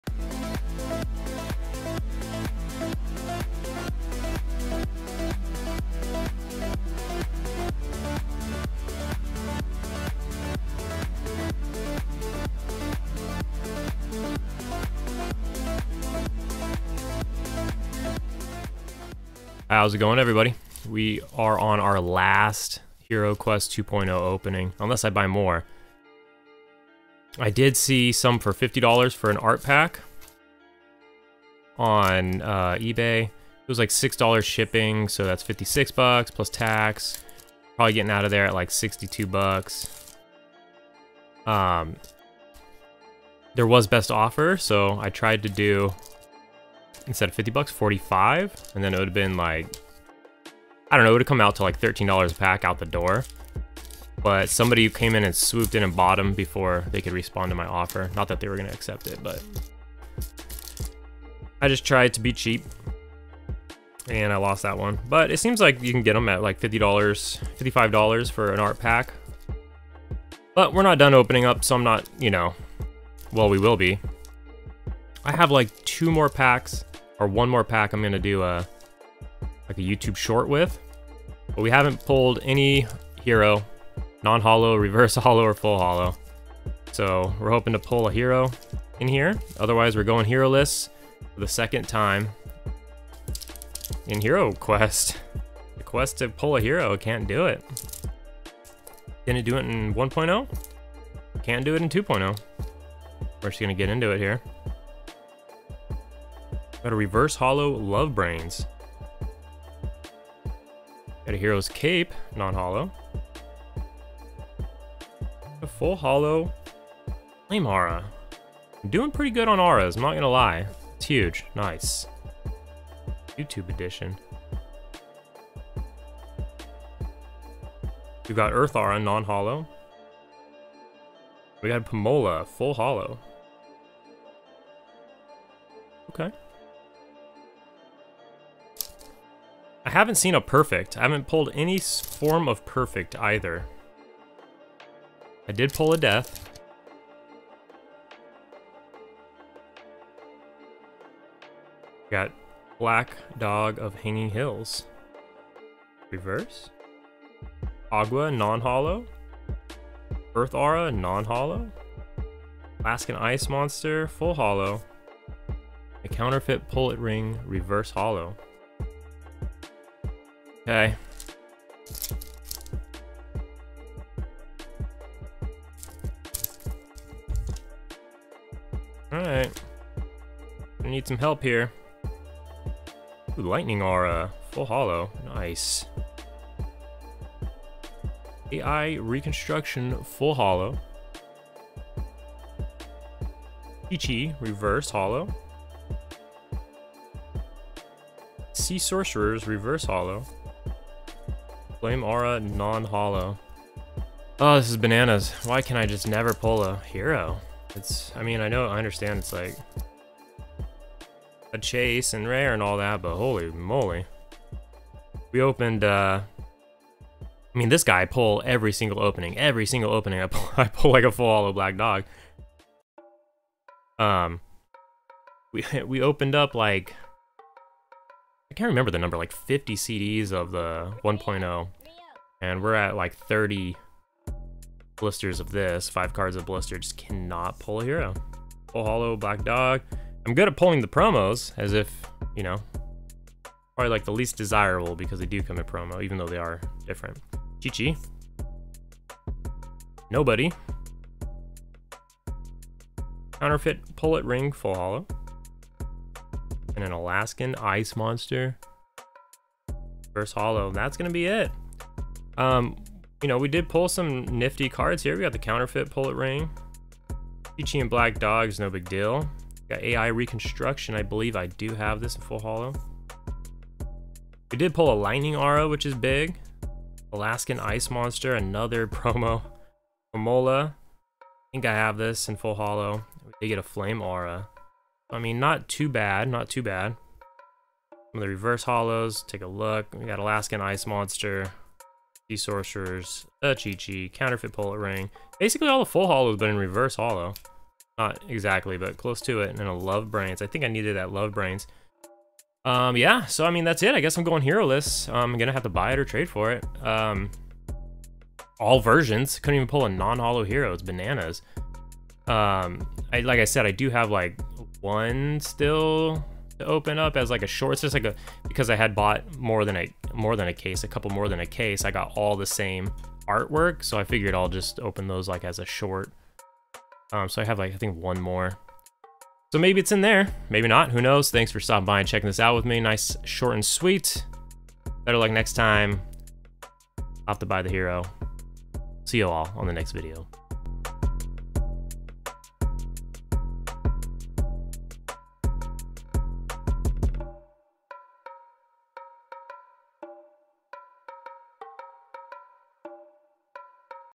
MetaZoo HiroQuest 2.0 Art Set Opening 6
In this video we open up an art set of HiroQest 2.0.

Chapters:

0:00 Intro
1:25 Pack Opening Begins
4:26 Recap
5:15 Outro